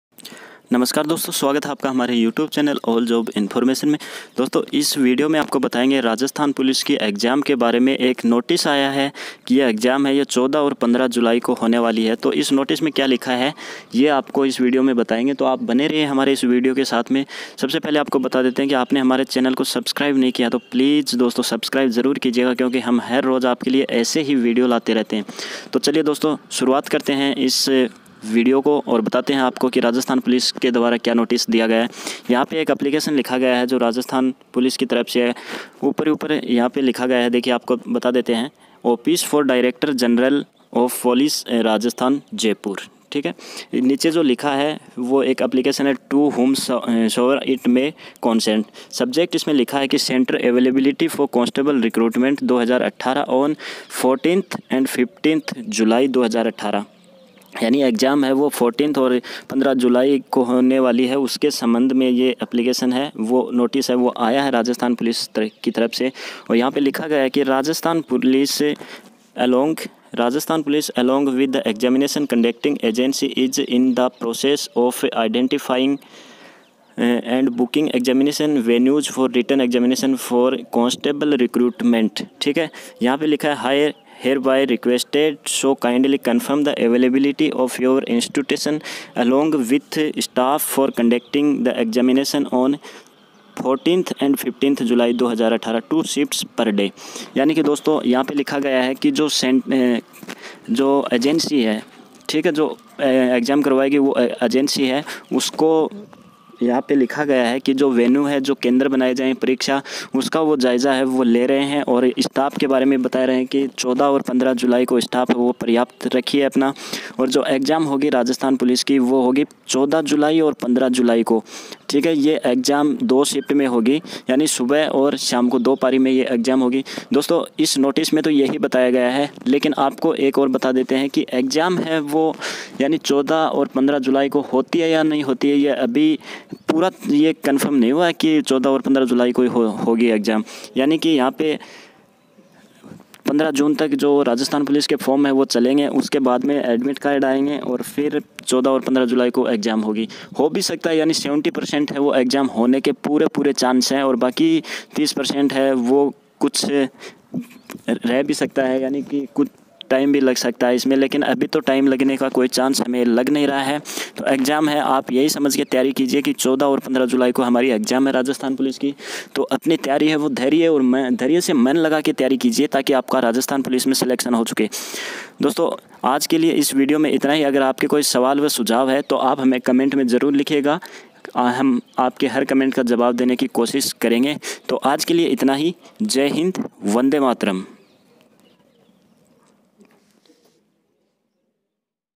Rajasthan Police constable bharti Exam Date Confirmed 14 Or 15 July
Hi friends aaj ke is video me hmne aapko Rajasthan police constable bharti ke exam ke baare me btaya hain jo ki dipartment ki traf se ek news jaari kiya gya h dosto asa karta hoon ye video aapko pasand aayega agar video aapko pasand aaye to video ko like kar de or share bhi kar diya karo dhanywad friends